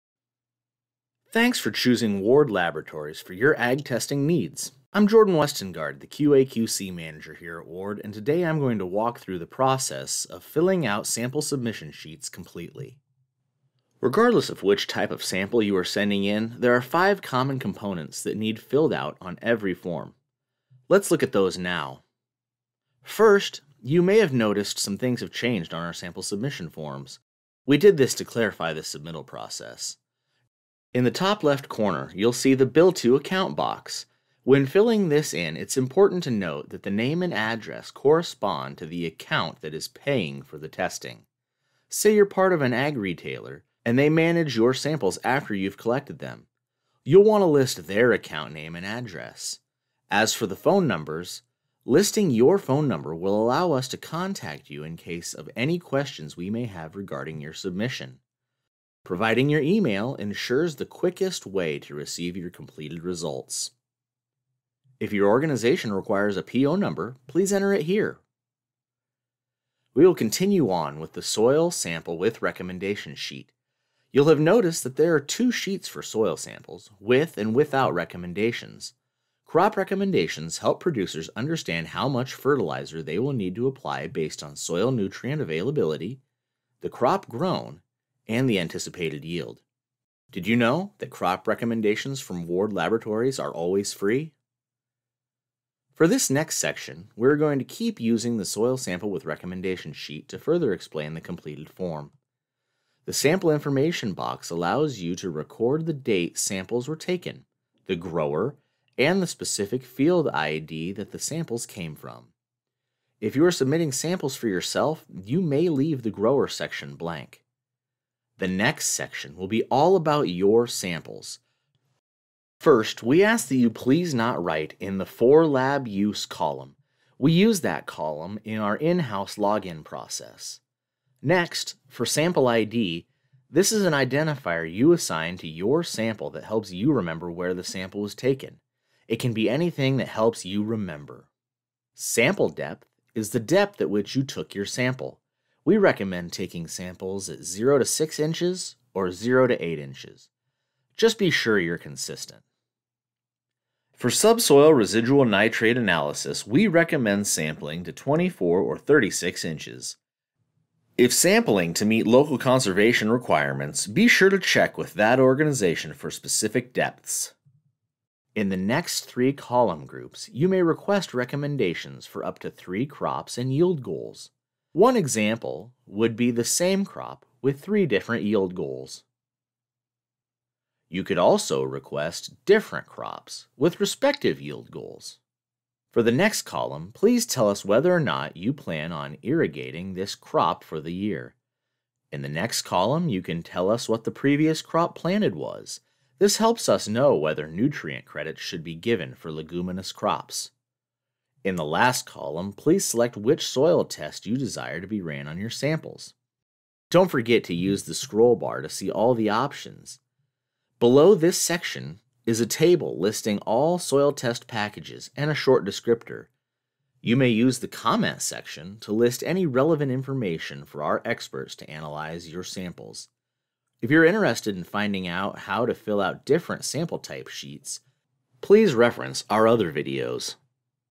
Filling Out a Soil Submittal Sheet
This video describes how to fill out a Ward Laboratories Soil Sample Submission Sheet.